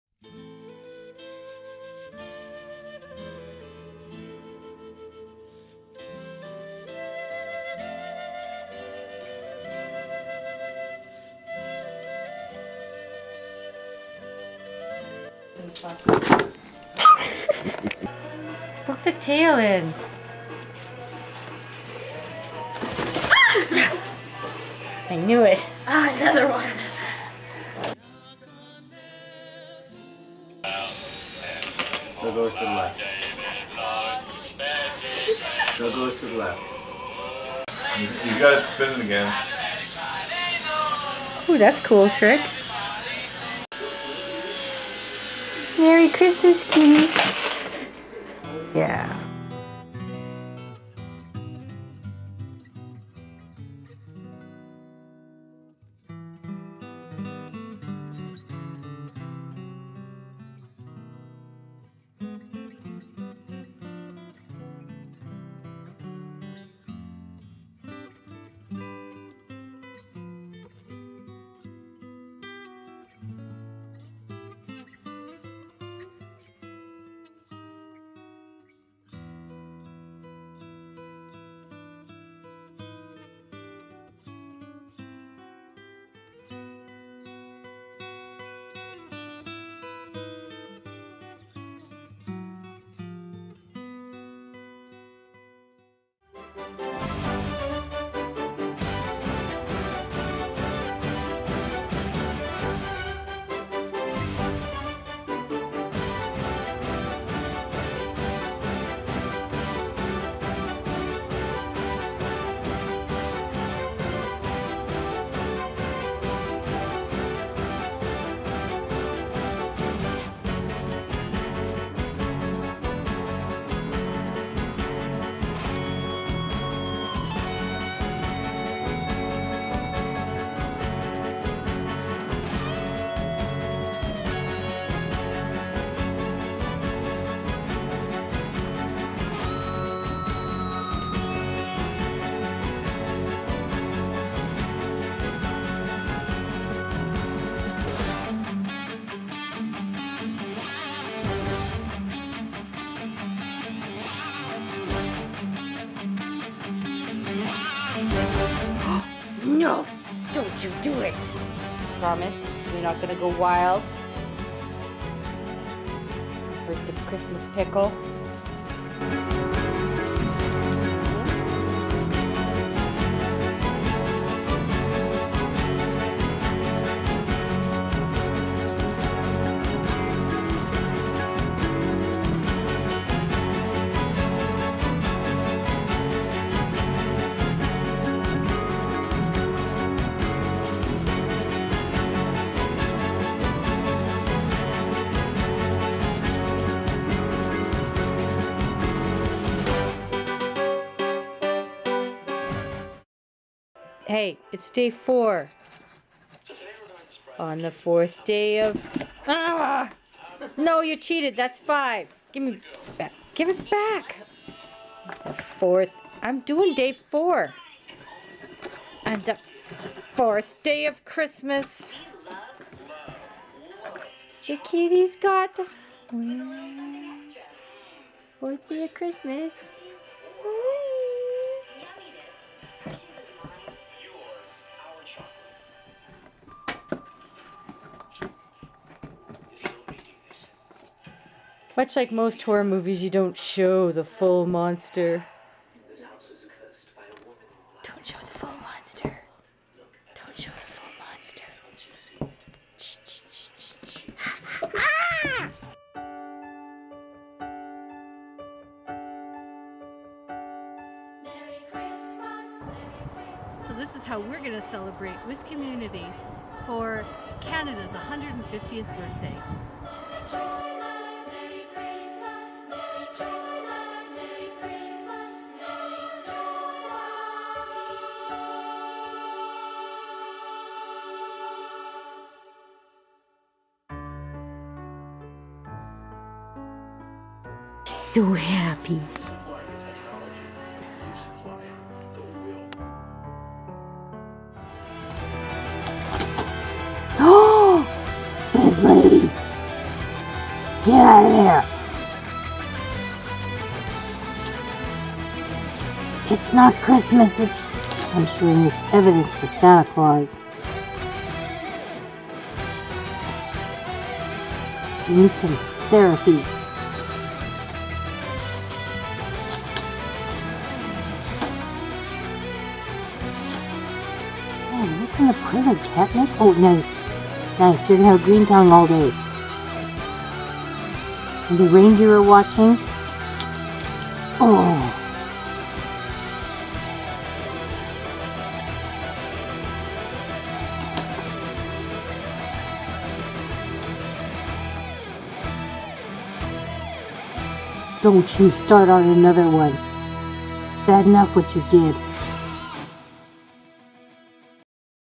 Cat Christmases - 6 years worth Fizzley Bubbley & Muchentuchen
Here is a history of 6 Cat Christmases. The 1st one was celebrated with a helicopter. We've had a Jurassic Christmas and a Harry Potter one. The cats have destroyed the tree and the gifts. They have eaten almost everything else. If you enjoy the antics of Russian Siberians, you'll have a belly laugh. There is even an Advent Calendar with cats behind the little doors. They cats have grey hair, dark circles under their eyes and they are cray. just like me!